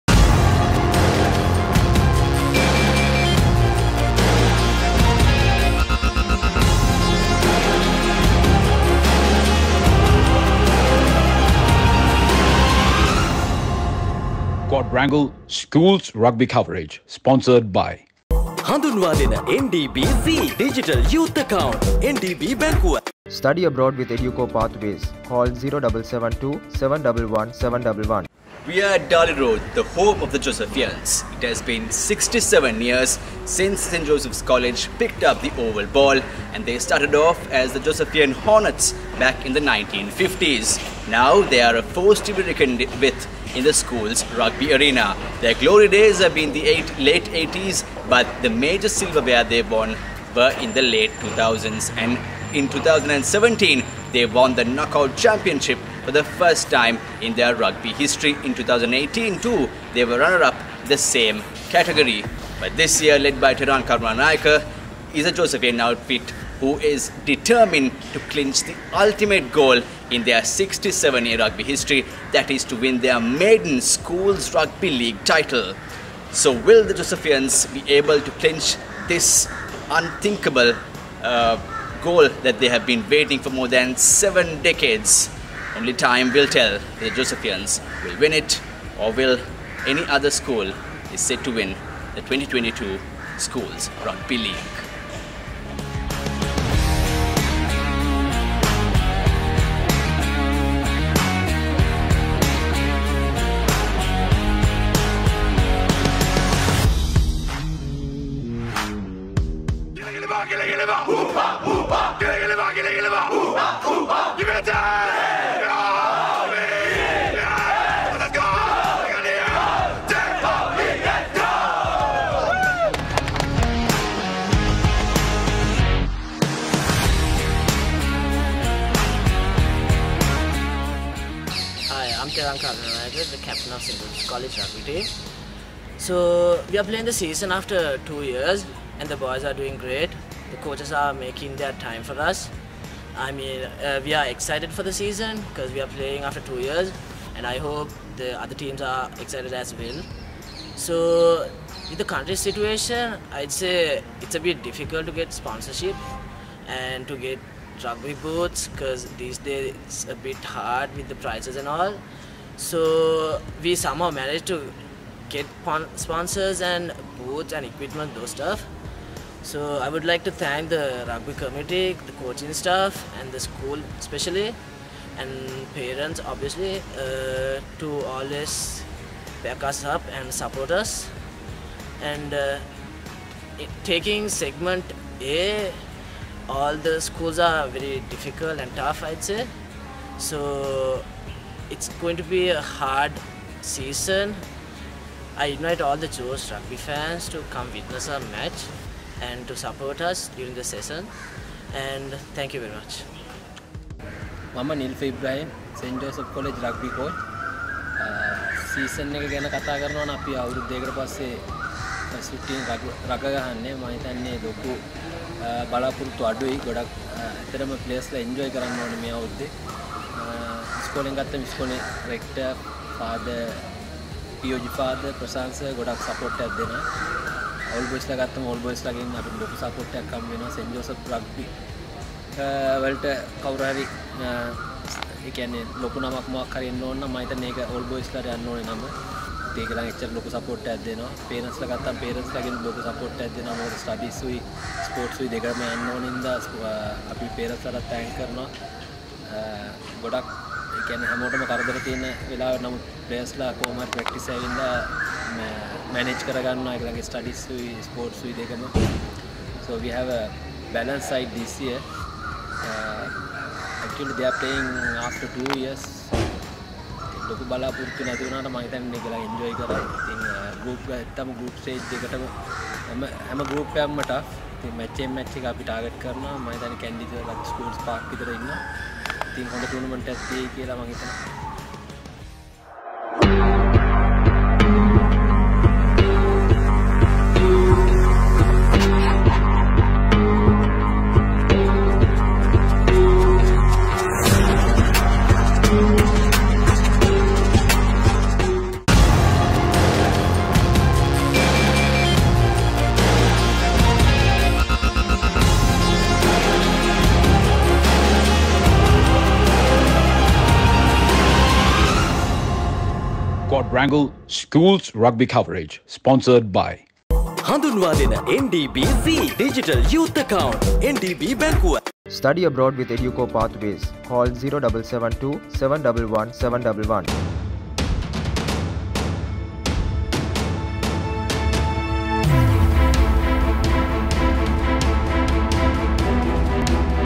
St.Joseph's College rugby - Preview 2022
Quadrangle Schools Rugby Coverage 2022 featuring St.Joseph's College 1st XV and their preparations for the upcoming season.